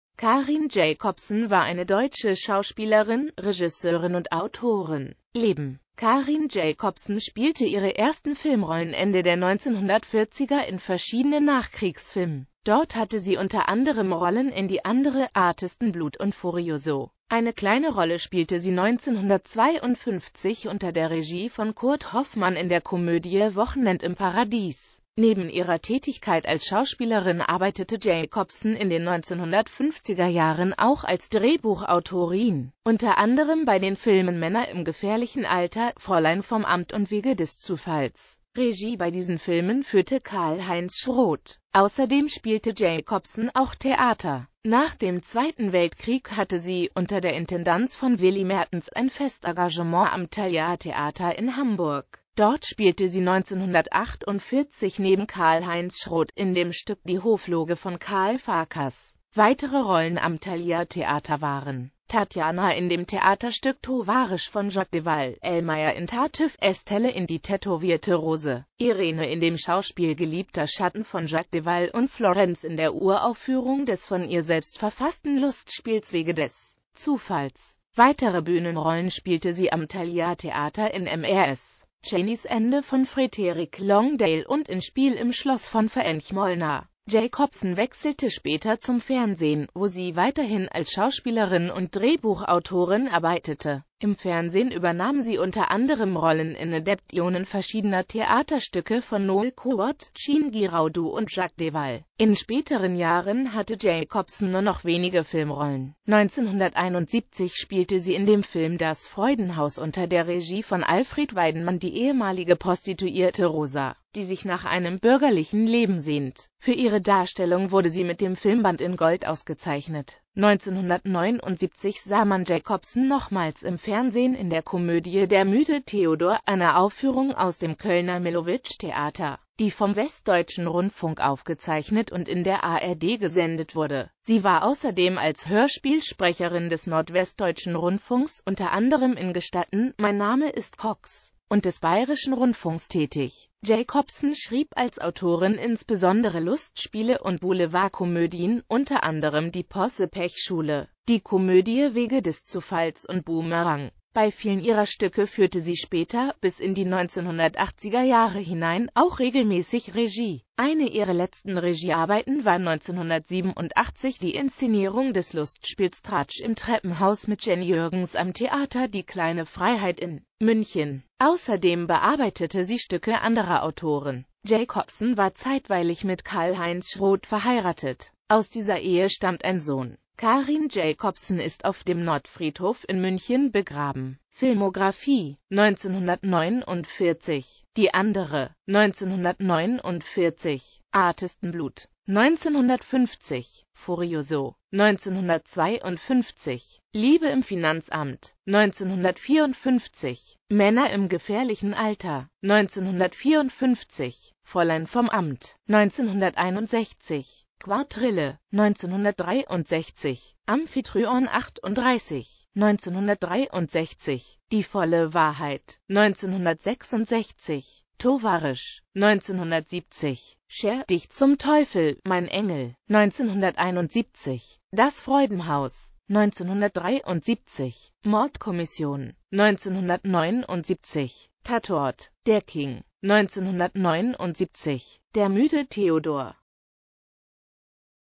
Karin Jacobsen
Vielen Dank für Ihre Unterstützung:

Karin Jacobsen (* 14.März 1924 in Düren; † 19.September 1989 in Nußdorf am Inn, Landkreis Rosenheim) war eine deutsche Schauspielerin, Regisseurin und Autorin.